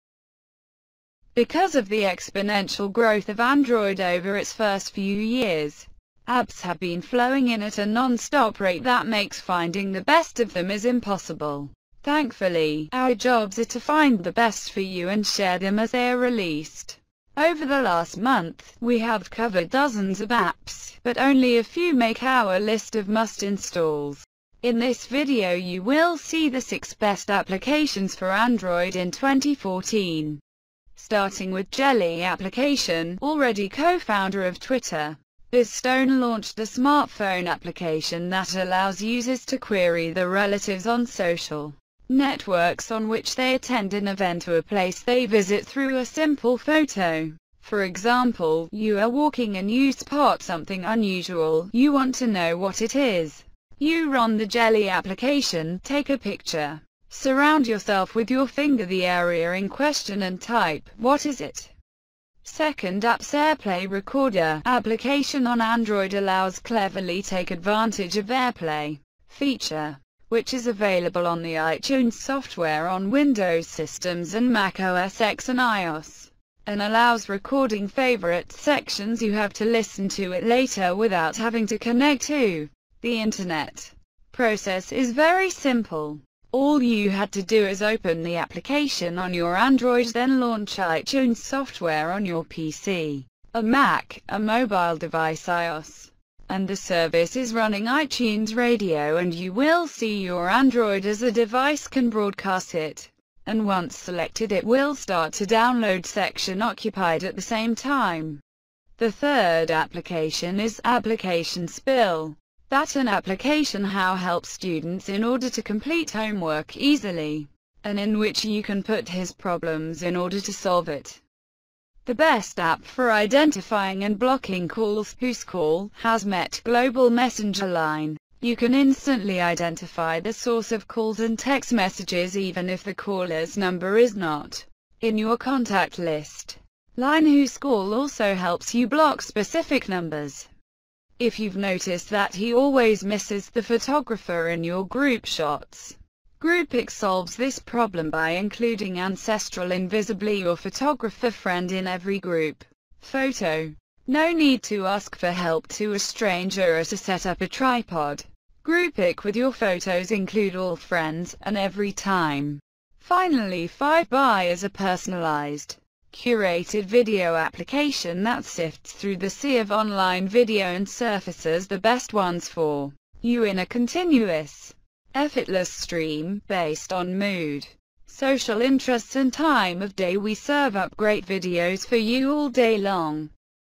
Best apps for Android 2014
What would smartphones, but for the application? A trip to the dining room, adventure booking tickets, the application will run applications dalje.Google play mammoth warehouse stores and you can always find someone who you have to get out of a difficult situation. Here is a list of the best new Android applications and Mars.